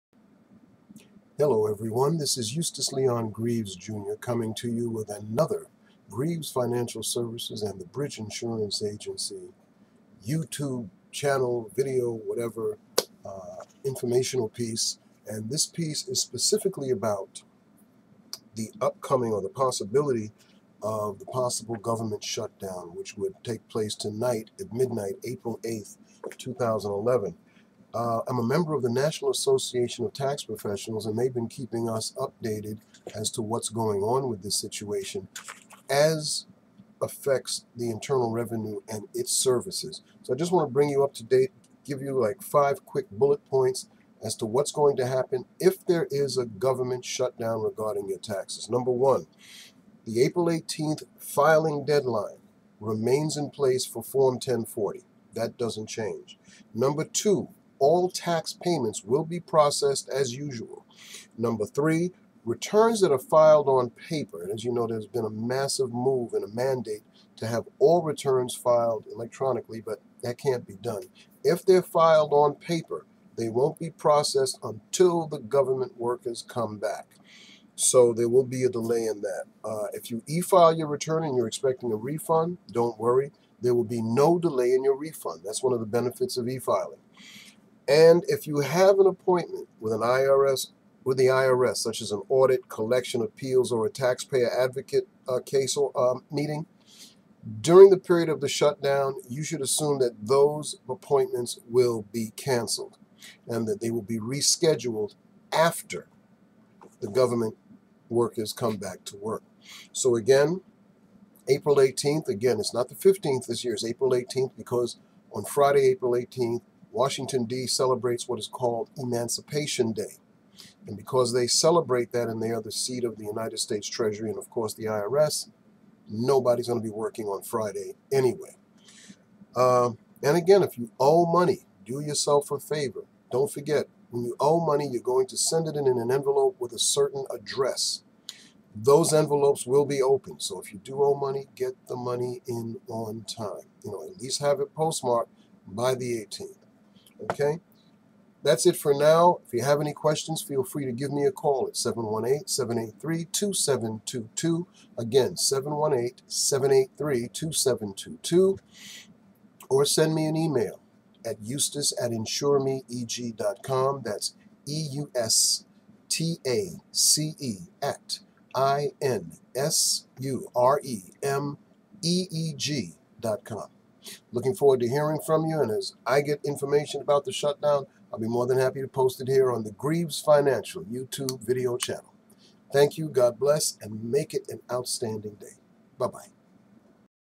Possible Gov't Shutdown effect on IRS
What are the IRS contingency plans in case of a government shutdown